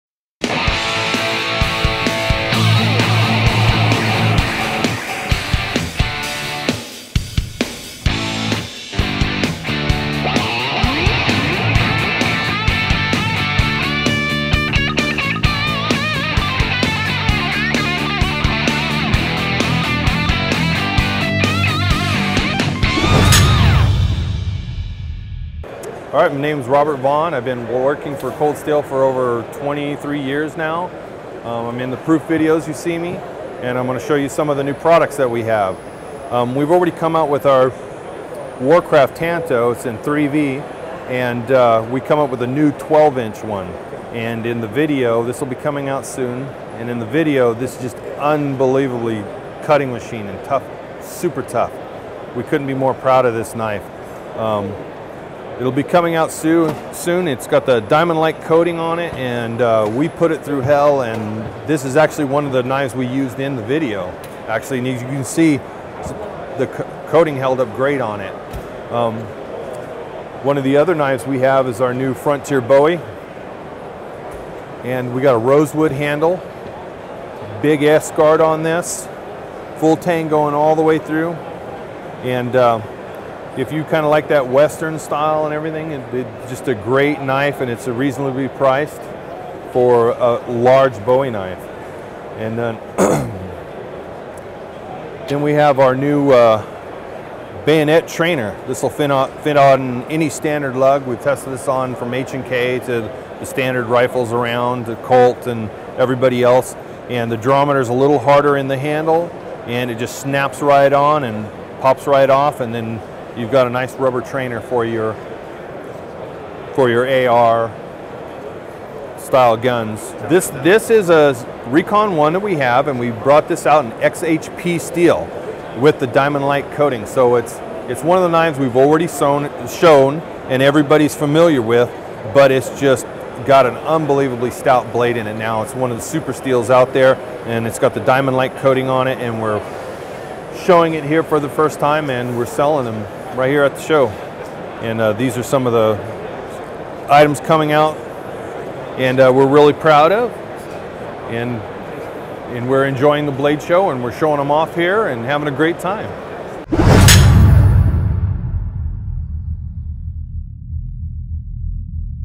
Blade Show 2014 Cold Steel
New Products Cold Steel Knives from Blade Show 2014.